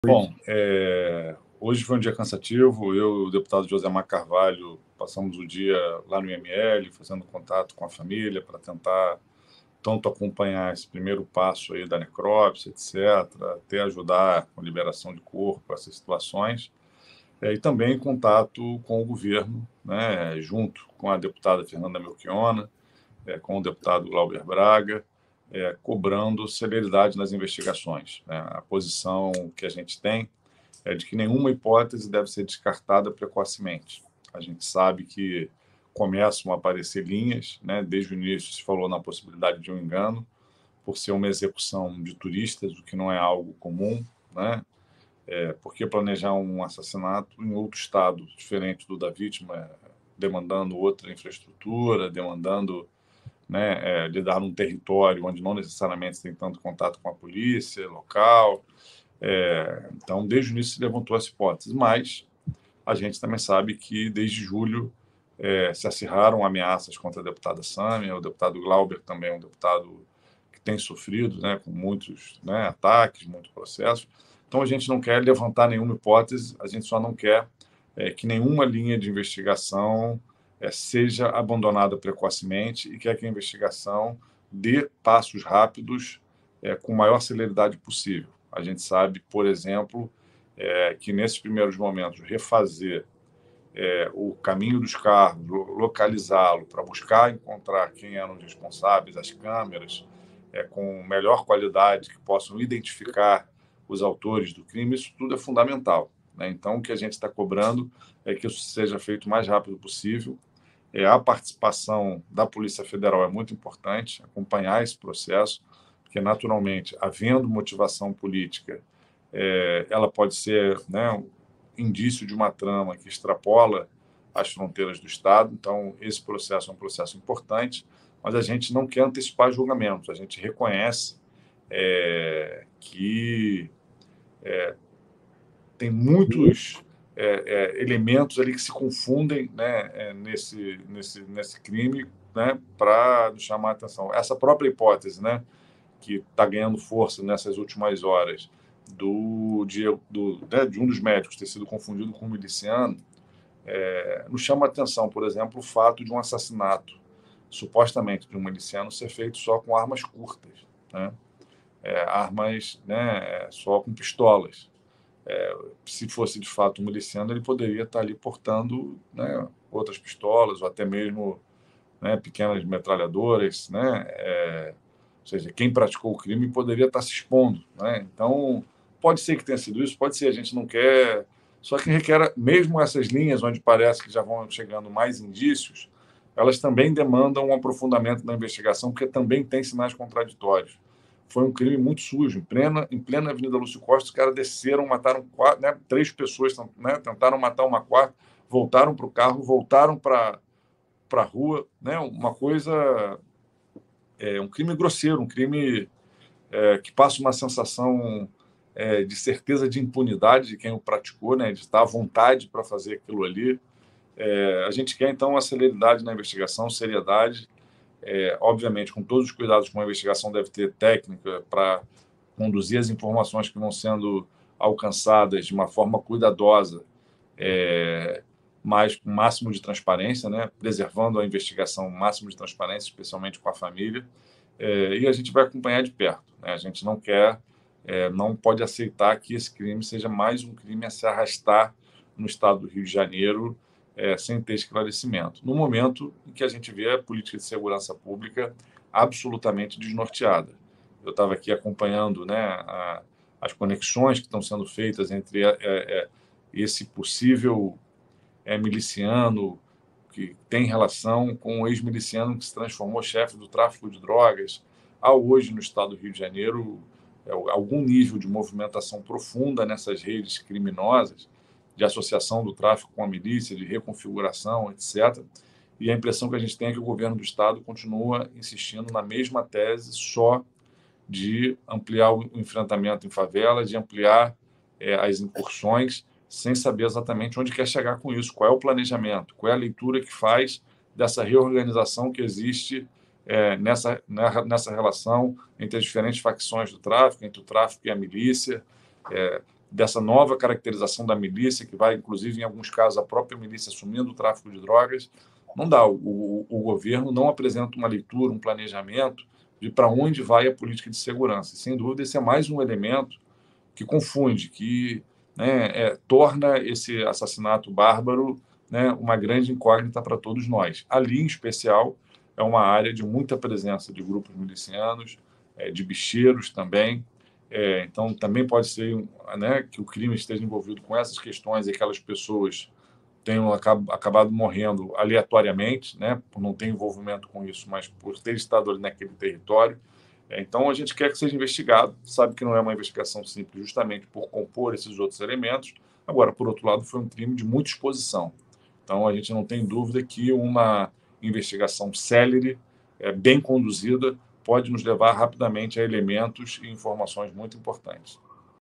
Deputado Estadual e Presidente do PSOL RJ descreve dia difícil com mortes dos médicos
00:00:00 - Nenhuma hipótese pode ser descartada